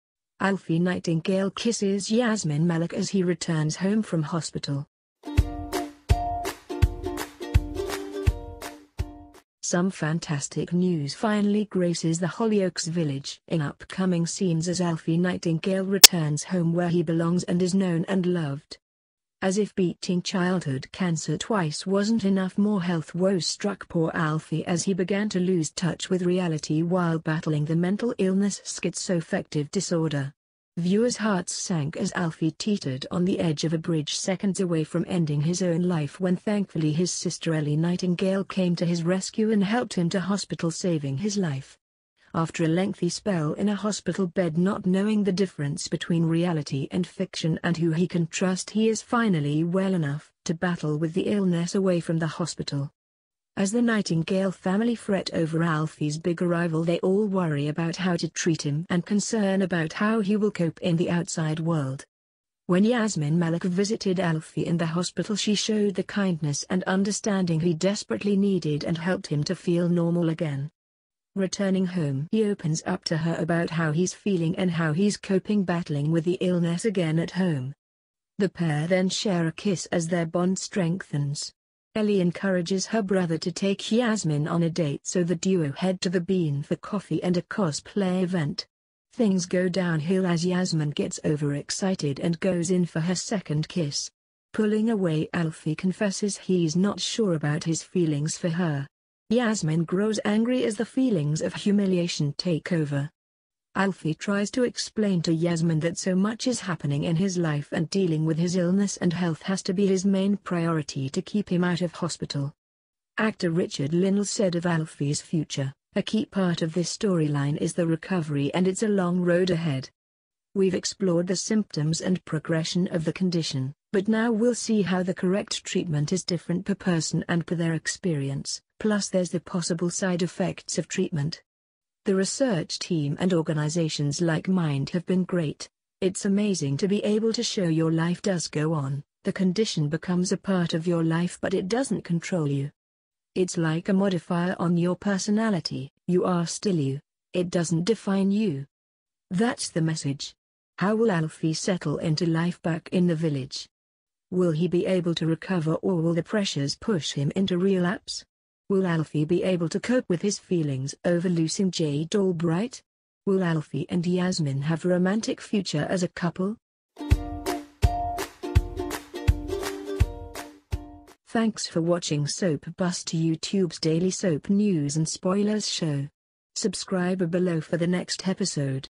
Hollyoaks ALFIE NIGHTINGALE Kisses And Dates YASMINE MAALIK As He Returns Home From Hospital!
Hollyoaks ALFIE NIGHTINGALE Kisses And Dates YASMINE MAALIK As He Returns Home From Hospital! Some fantastic news finally graces the Holly oaks village in upcoming scenes as Alfie Nightingale returns home where he belongs and is known and loved. As if beating Childhood Cancer twice wasn't enough more Health woes struck poor Alfie as he began to lose touch with reality while battling the Mental Illness Schizoaffective Disorder. Viewers hearts sank as Alfie teetered on the edge of a Bridge seconds away from ending his own life when thankfully his Sister Ellie Nightingale came to his rescue and helped him to Hospital saving his life. After a lengthy spell in a Hospital Bed not knowing the difrence between reality and fiction and who he can trust he is finally well enough to battle with the illness away from the Hospital. As the Nightingale family fret over Alfie's big arrival they all worry about how to treat him and concern about how he will cope in the outside world. When Yasmine Maalik visited Alfie in the Hospital she showed the kindness and understanding he  desperately needed and helped him to feel normal again. Returning home he opens up to her about how he’s feeling and how he's coping battling with the illness again at home. The pair then share a kiss as their bond strengthens. Ellie encourages her Brother to take Yasmine on a date so the duo head to the Bean for Coffee and a cos-play event. Things go down hill as Yasmine gets over excited and goes in for her second kiss. Pulling away Alfie confesses he's not sure about his feelings for her. Yasmine grows angry as the feelings of humiliation take over. Alfie tries to explain to Yasmine that so much is happening in his life and dealing with his illness and health has to be his main priority to keep him out of hospital. Actor Richard Linnell said of Alfie's future: “A key part of this storyline is the recovery and it’s a long road ahead. We’ve explored the symptoms and progression of the condition, but now we’ll see how the correct treatment is different per person and per their experience, plus there’s the possible side effects of treatment. The research team and organisations like Mind have been great, it’s amazing to be able to show your life does go on, the condition becomes a part of your life but it doesn’t control you. Its like a modifier on your personality, you are still you – it doesn’t define you. That’s the message.” How will Alfie settle into life back in the village? Will he be able to recover or will the pressures push him into relapse? Will Alfie be able to cope with his feelings over loosing Jade Albright? Will Alfie and Yasmine have a romantic future as a couple?